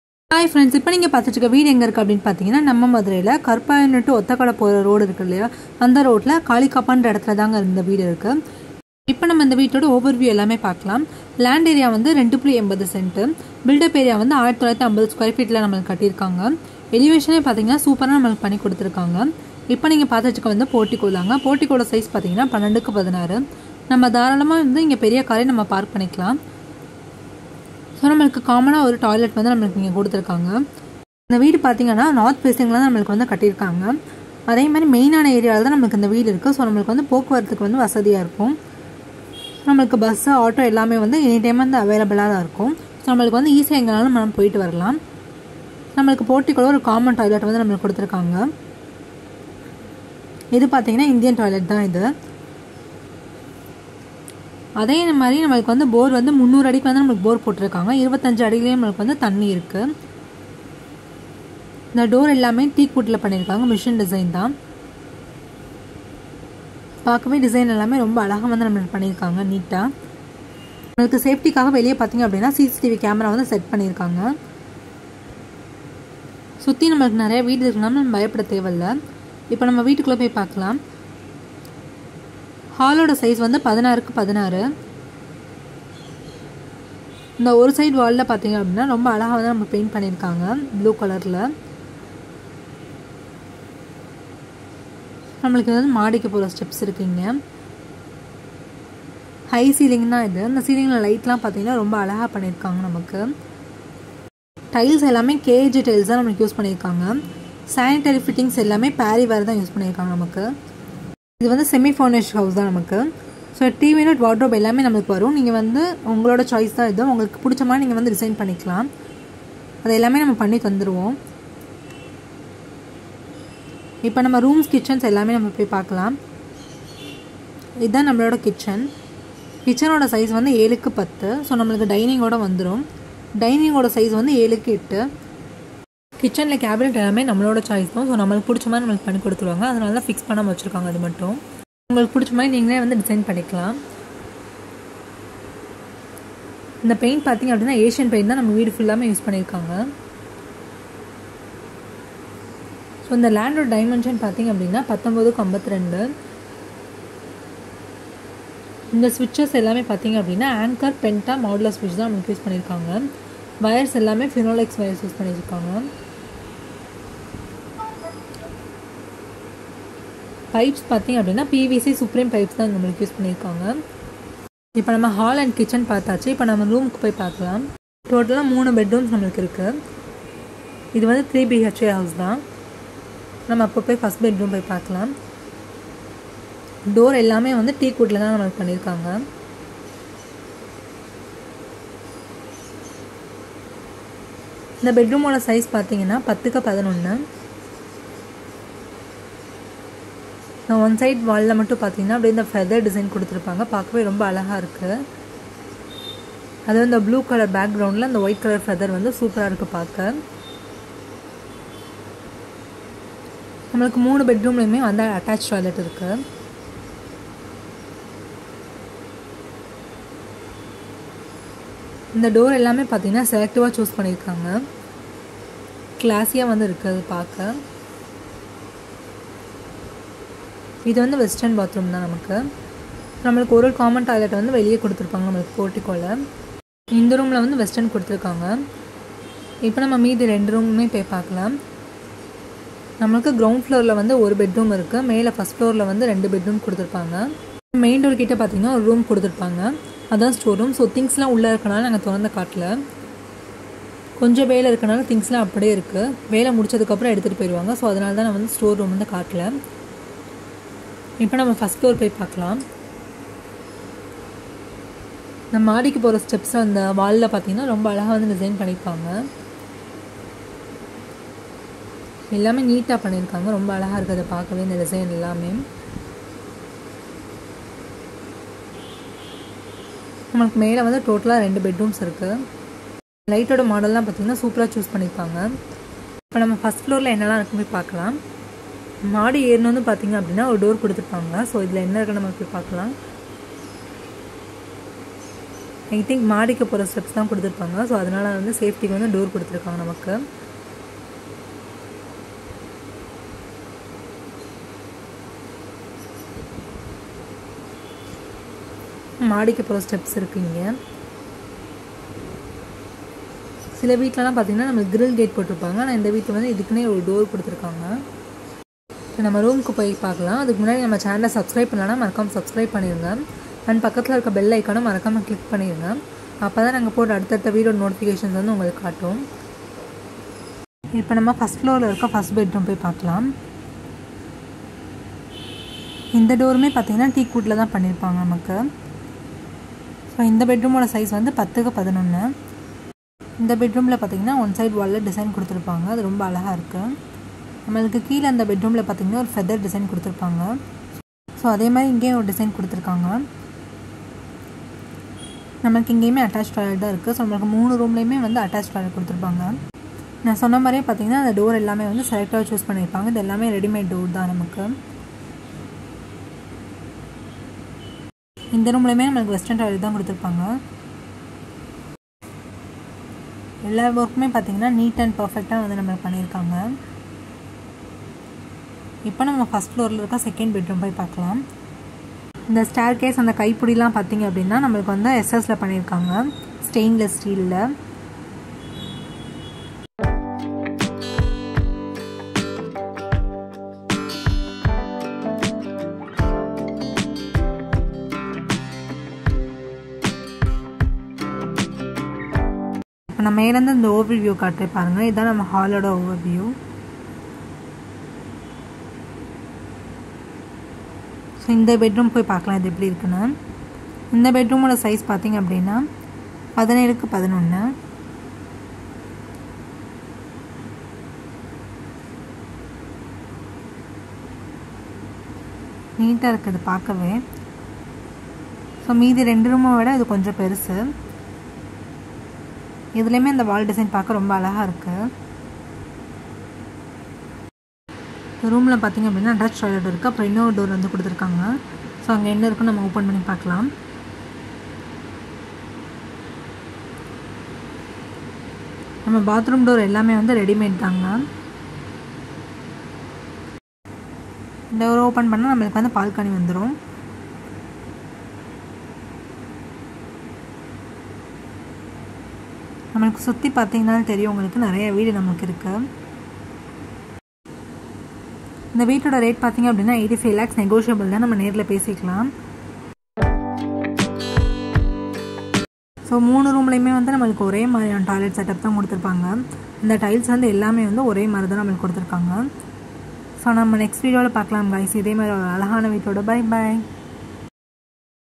CCTV|CAR PARKING|3BHK| MODULAR KITCHEN|TVUNIT| WARDROBE|HIGH CEILING VILLA IN KARUPPAYURANI
Kalikappan
North facing
Neat location.
3 Bedrooms Size
      10' x 11'- 2No's
      17' x 11'- 1No
Hall Size
      16' × 16'
Kitchen size
       7' x 10'
Dining
       7' x 8'
Parking
       12' x 16'
Store room
        4' × 7'
3 attached bathroom
Size.    7' × 4'
Common bathroom size
9' × 4'

Borewell- 300 ft with Texmo submerged pumb(water level-20ft)
Septic tank-5500 lt capacity with choke pit.
Brick- Apthagir wire cut brick.
Cement- chettinad & jsw.
Steel- Ishwaryam 550d.
Switches-Anchor Penta modular switch.
Wires- Finolex.
Pipes : PVC-Supreme
CPVC-Ashirvad.
Water tank-Meerakunj.
Tiles- KAG tiles.
Sanitary fittings- Parryware.
Inside and outside handrills- SS 304 grade.

We provide
Attached saftey camera.

Wardrobe and modular kitchen will be fixed based on client requirements.